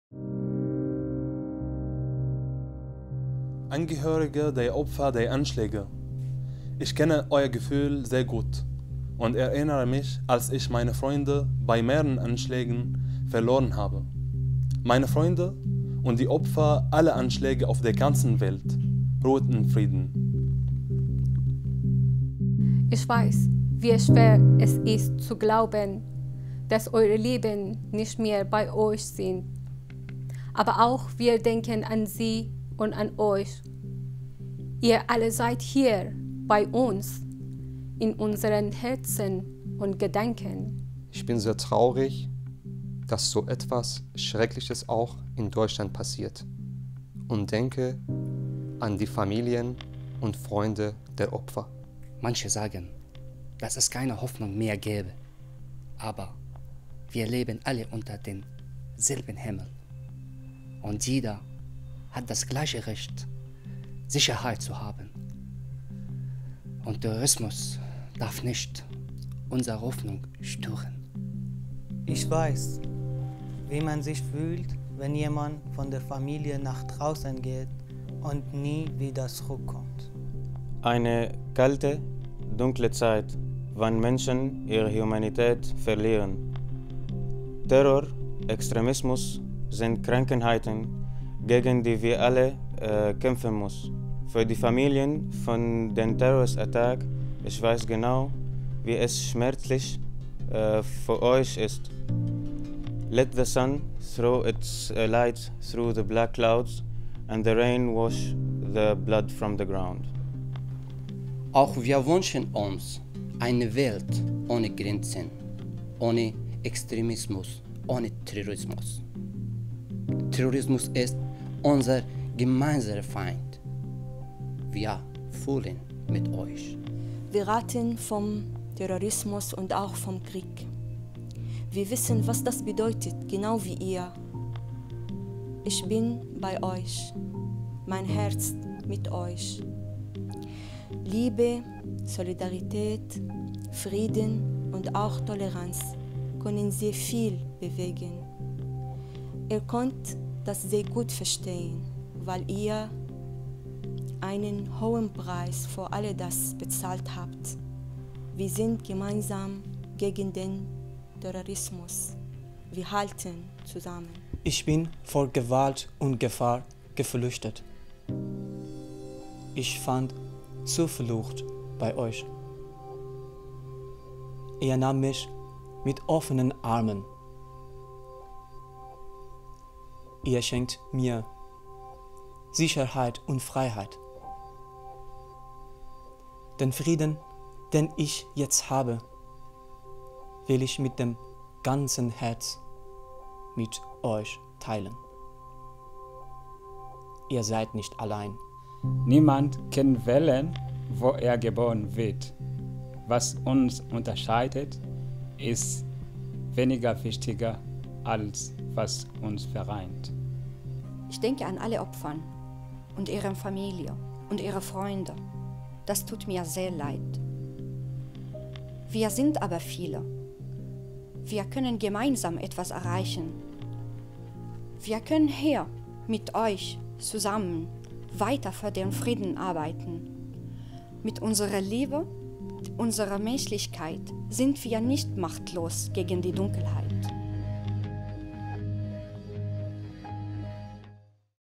Compassion [Mitgefuehl]
This four-minute video has been produced in 2016 by participants of the trainee programme for media builders in Exile at Hamburg Media School after the hate crimes in Germany - but can be read as a statement against terrorist attacks and hate crime worldwide.
In german language with english subtitles.

If you agree, please spread the link with the Hashtag #'Compassion or Mietgefuehl.

© Hamburg Media School / 'Digital Media for Refugees' [DMF] The continuing education program of the HMS for media crews with a history of escape.

The video was recorded in the TV studio of TIDE, Hamburg's community broadcasting channel / station - radio & tv.

By and with
Ahmad Al-Rifaee, Zahra Sadat, Adam Frei, Aamer Najjar, Arman Ahmadi, Maher Alkadi, Hamid Saljooghi, Rosa Yassin Hassan, Jalal Hosseini, Jean Ngendahimana and Roshak Ahmad.
Other contributors: Tina Fritsche, Christiane Buchmann, Christian Frank & Andreas Bruhn and Prof. Richard Reitinger.
English Subtitels: Sally Johnson.